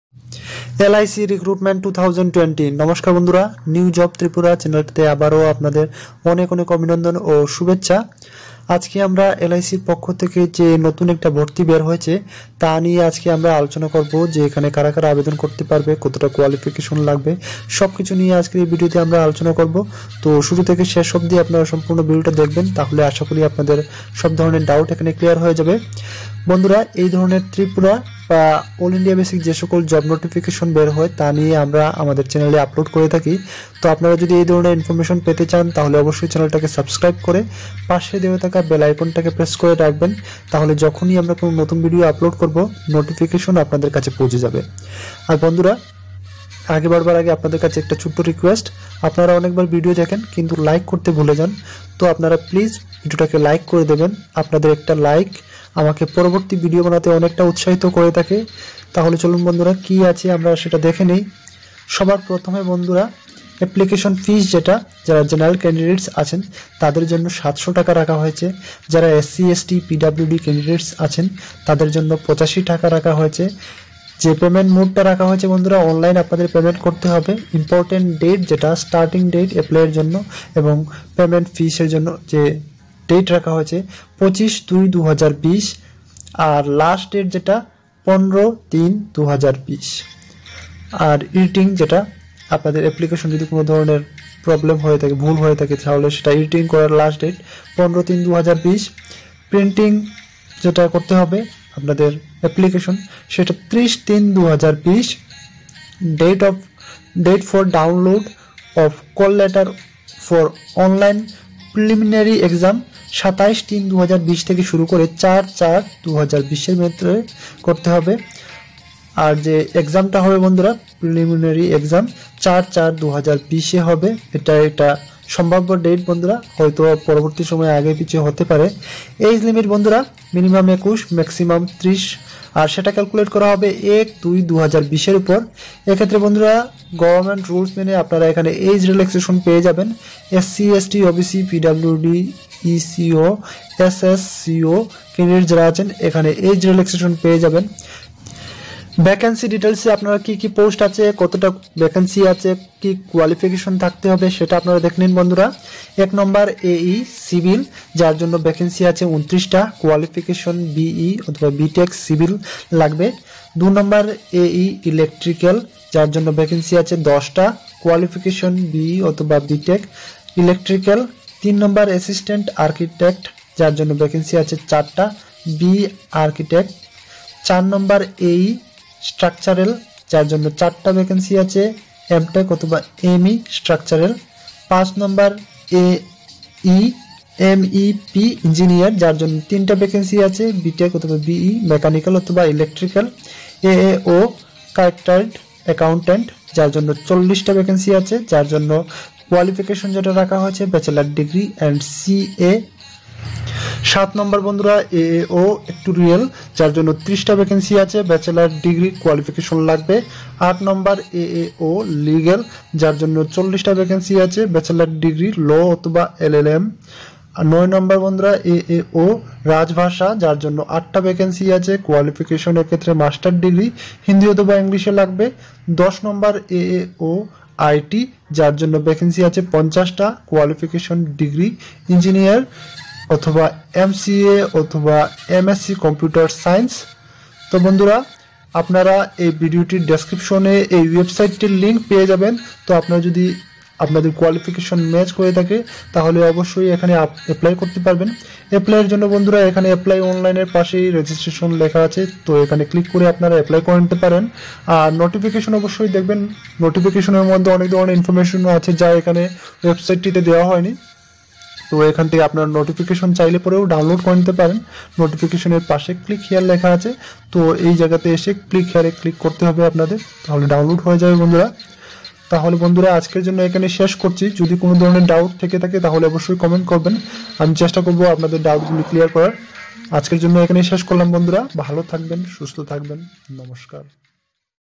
Tripura Job News 2020#4 || NewJob
Tripura Job News 2020 || Tripura Job || NewJob

: Life Insurance Corporation of India (LIC) has published notification for the recruitment of Assistant Engineer (AE) – Civil/ Electrical /Structural /MEP & Assistant Architect (AA) and Assistant Administrative Officer (AAO) Specialist vacancies. Those Candidates who are interested in the vacancy details & completed all eligibility criteria can read the Notification & Apply Online.
Life Insurance Corporation of India (LIC)
AE & AAO Vacancies 2020
Application Fee

For Others: Rs.700/- (Application Fee-cum-Intimation Charges + Transaction Charges + GST)

For SC/ST/PWD candidates: Rs.85/- (Intimation Charges + Transaction Charges + GST)

Payment Mode: Through Online

Important Dates

Starting Date to Apply Online & Payment of Fee/  Intimation Charges: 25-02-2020

Last Date to Apply Online & Payment of Fee/  Intimation Charges: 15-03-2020

Last Date for Editing Application Details: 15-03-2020

Last Date for Printing Your Application: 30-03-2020

Date for Download of Call Letter for Online Preliminary Exam: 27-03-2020 to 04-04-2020

Tentative Date for Prelims Exam: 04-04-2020

tripura job news 2020,
tripura job,
tripura govt job 2020,
tripura job news,
tripura news,
tripurar sarkari chakrir khabor,